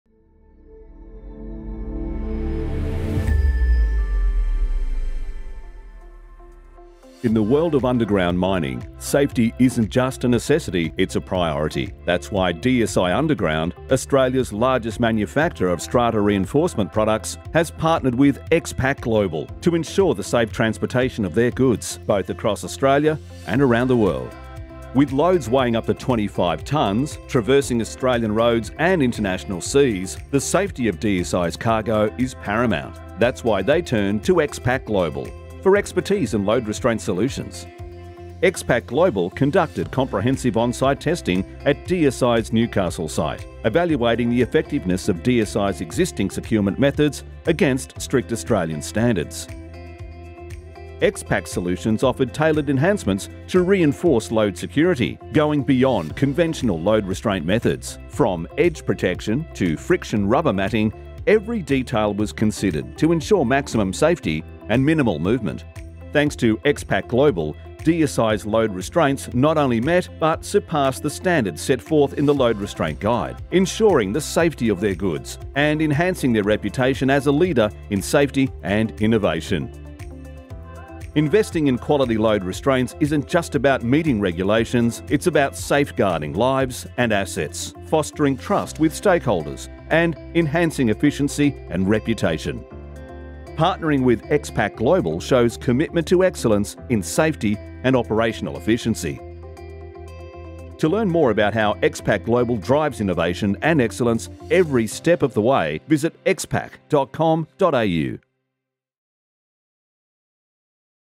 How DSI Underground and X-Pak Global Ensure Safety & Security on the Road
In the world of underground mining, safety isn't just a necessity—it's a priority. That's why DSI Underground, Australia's largest manufacturer of strata reinforcement products, partnered with X-Pak Global to ensure the safe transportation of their goods, both across Australia and around the world.
With loads weighing up to 25 tonnes traversing Australian roads and international seas, the safety of DSI's cargo is paramount. That's why they turned to X-Pak Global for expertise in load restraint solutions.
X-Pak Global conducted comprehensive on-site testing at DSI's Newcastle site, evaluating the effectiveness of DSI's existing securement methods against strict Australian standards.
X-Pak's solutions offered tailored enhancements to reinforce load security, going beyond conventional load restraint methods. From edge protection to friction rubber matting, every detail was considered to ensure maximum safety and minimal movement.
Thanks to X-Pak Global, DSI's load restraints not only met but surpassed the standards set forth in the Load Restraint Guide, ensuring the safety of their goods and enhancing their reputation as a leader in safety and innovation.
Investing in quality load restraints isn't just about meeting regulations—it's about safeguarding lives and assets, fostering trust with stakeholders, and enhancing efficiency and reputation.
Partnering with X-Pak Global shows commitment to excellence in safety and operational efficiency. To learn more about how X-Pak Global drives innovation and excellence every step of the way